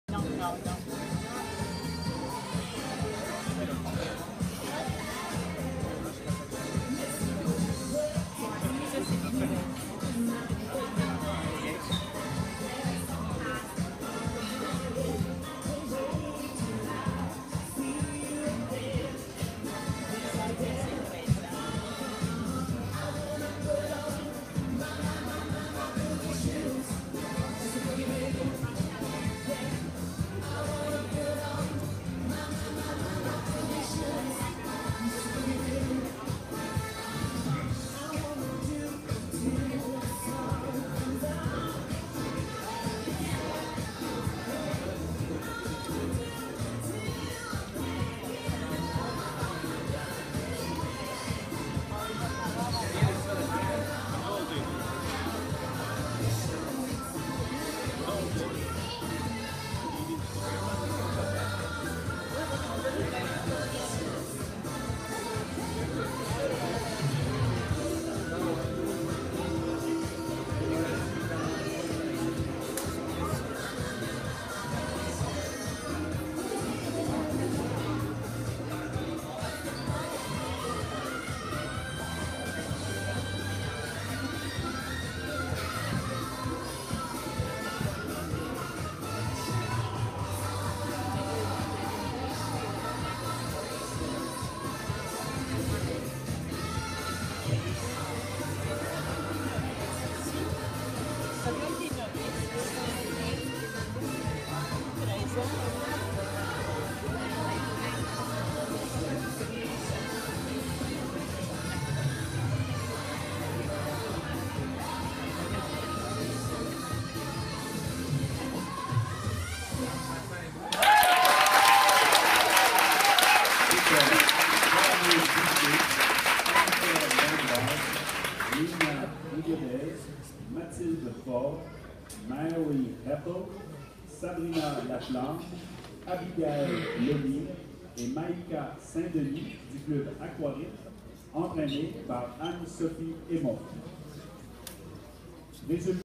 Nage synchronisée championnat provincial 22 mars 2015
Équipe Aquarythme
novice 13 ans et plus